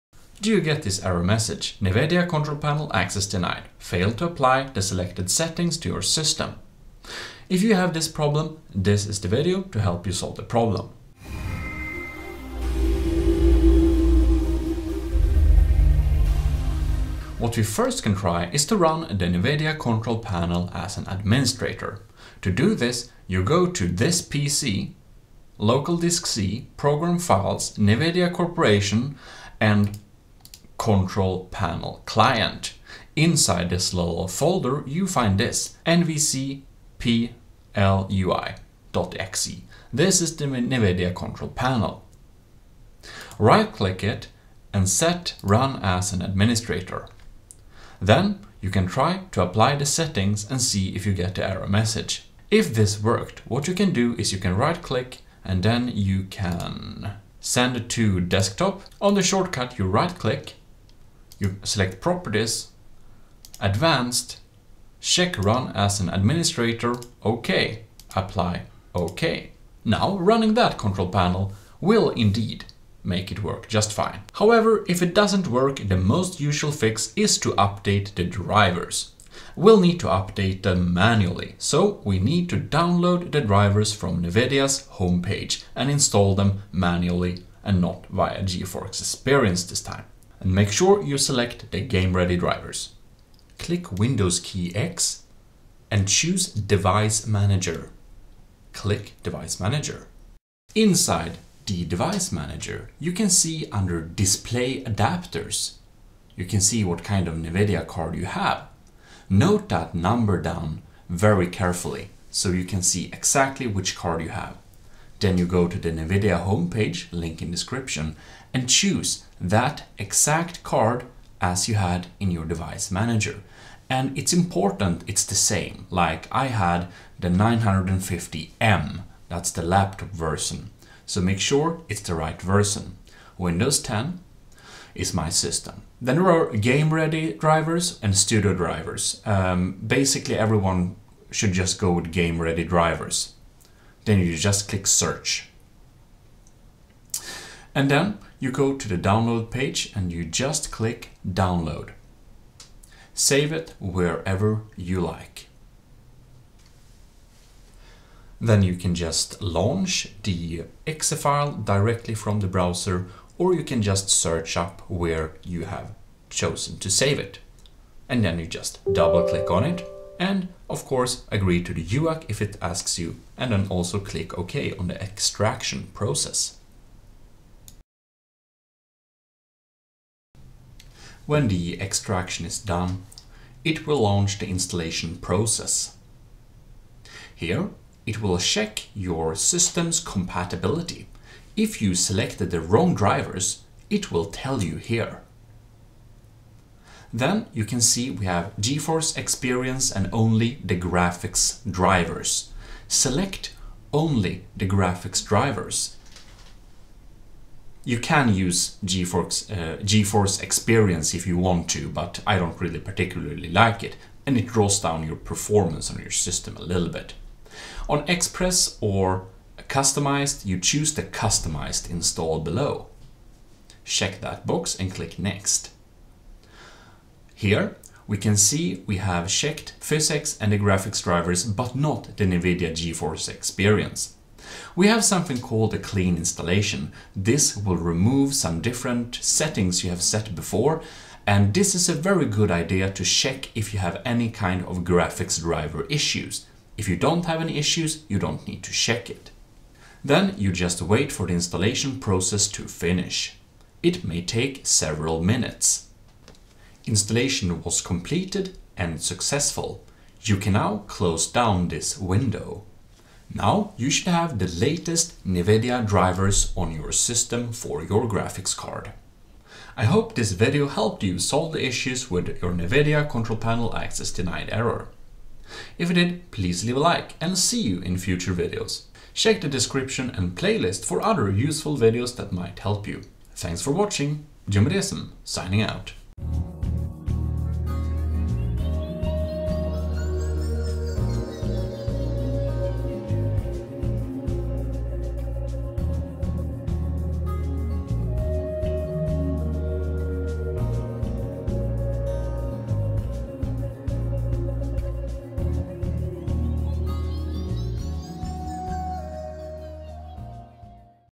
How to Fix: "Failed to apply selected settings to your system" - NVIDIA Control Panel Access Denied
How to fix failed to apply selected settings to your system  in NVIDIA Control Panel, Access Denied.
This youtube video explains how you can fix this issue and solve your Nvidia GPU problems and error messages. This fix will work on Windows 10, Windows 8 and Windows 7.

║ CHECK MY OTHER TECH TUTORIALS

Licensed to me according to our agreements.
T4W: How to Fix: "Failed to apply selected settings to your system" - NVIDIA Control Panel Access Denied